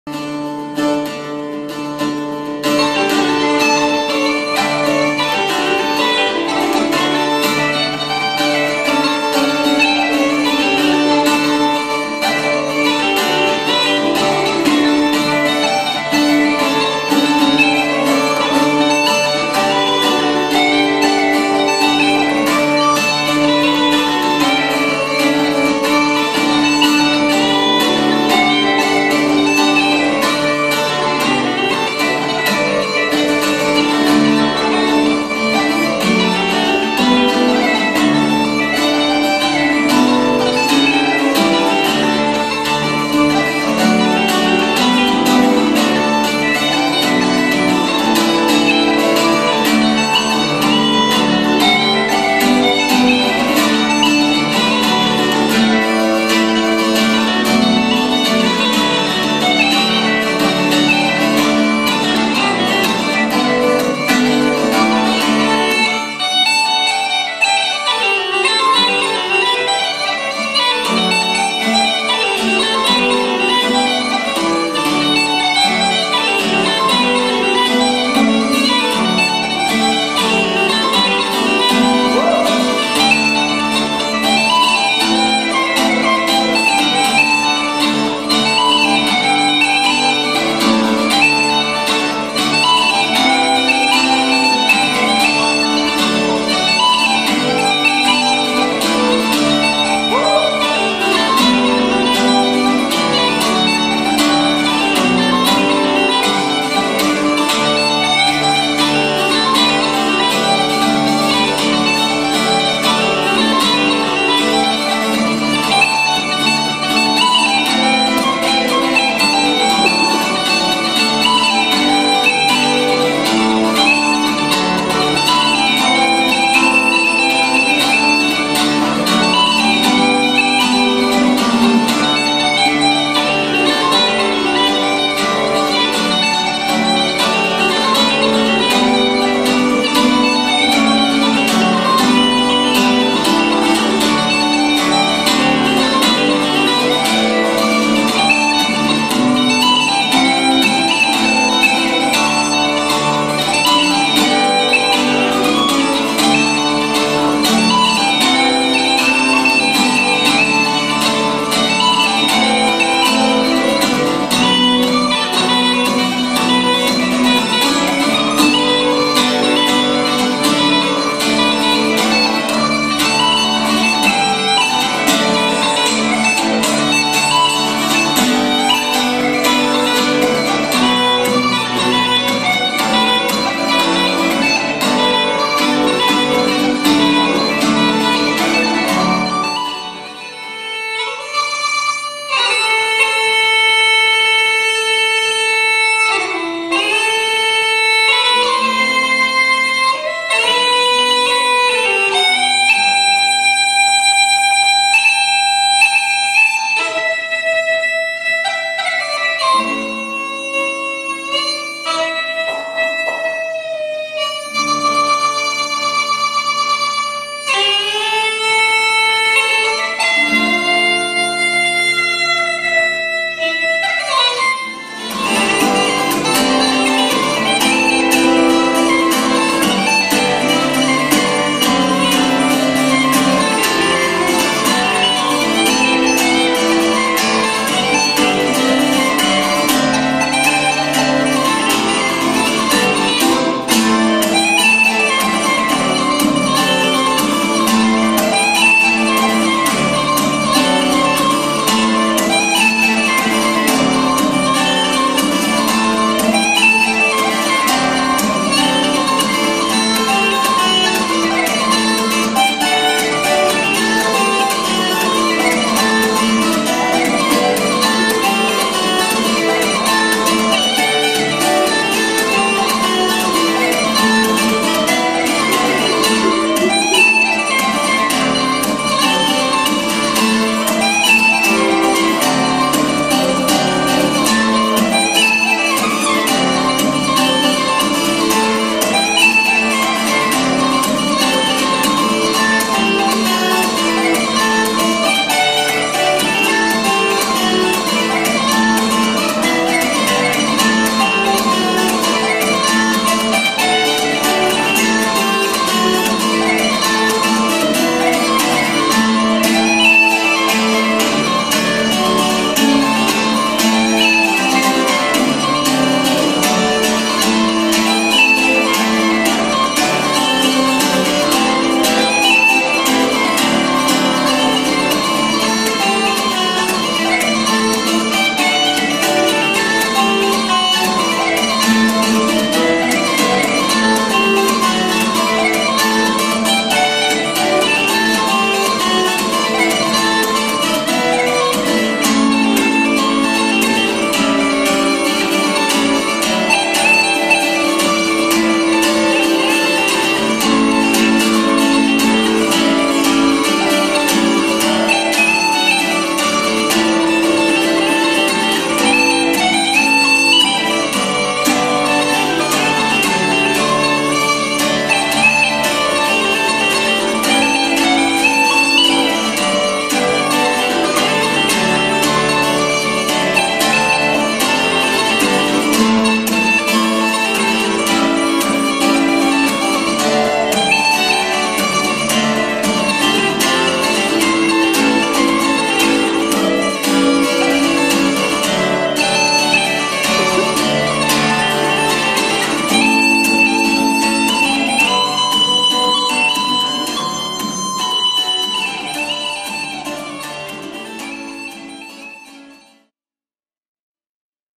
Geza Frank & Jean Damei Uilleann Pipes / Guitar - 2014
A few outtakes from a concert in Bad St. Leonhard, Austria, in 2014